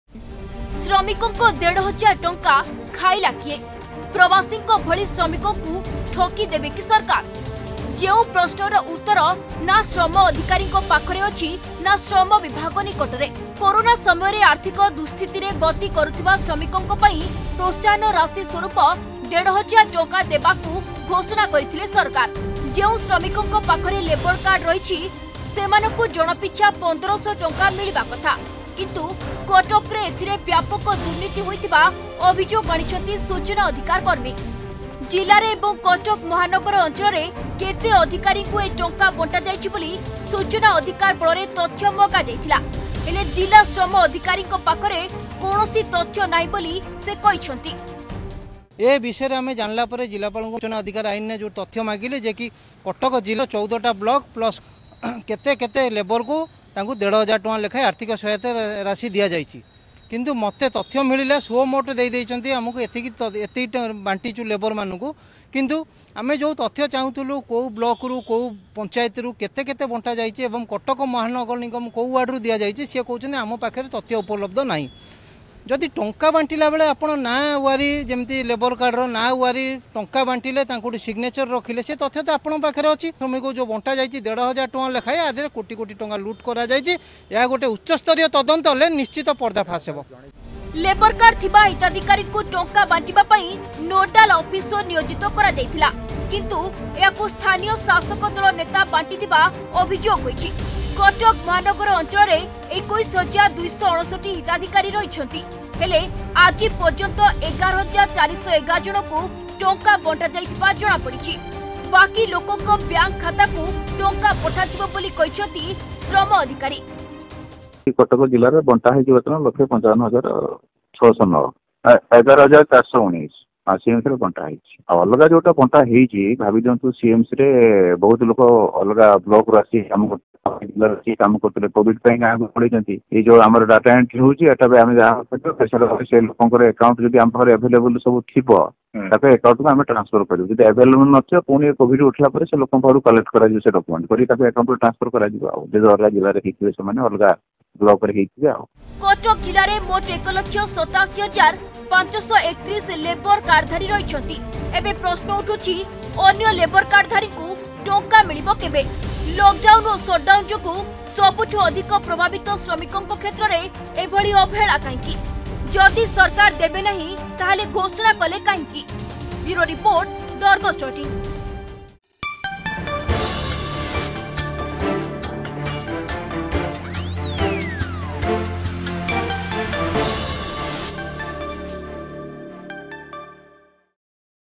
ପ୍ରବାସୀଙ୍କ ଭଳି ଶ୍ରମିକଙ୍କୁ ଠକିବାକୁ ଯୋଜନା .....ଶ୍ରମିକଙ୍କ ଦେଢ ହଜାର ଟଙ୍କା ଖାଇଲା କିଏ ?
କୋରନା ପାଇଁ ଶ୍ରମିକଙ୍କୁ ପ୍ରୋତ୍ସାହନ ରାଶି ଭାବେ ଦେଢ ହଜାର ଟଙ୍କା ଦେବକୁ ଘୋଷଣା କରିଥିଲେ ସରକାର । ହେଲେ ଏଥିରେ ବ୍ୟାପକ ଦୁର୍ନୀତି ହୋଇଥିବା ଅଭିଯୋଗ ଆଣିଛନ୍ତି ସୂଚନା ଅଧିକାର କର୍ମୀ । ନା ଅଧିକାରୀଙ୍କ ପାଖରେ ତଥ୍ୟ ନା ଶ୍ରମ ବିଭାଗ ପାଖରେ ଉତ୍ତର .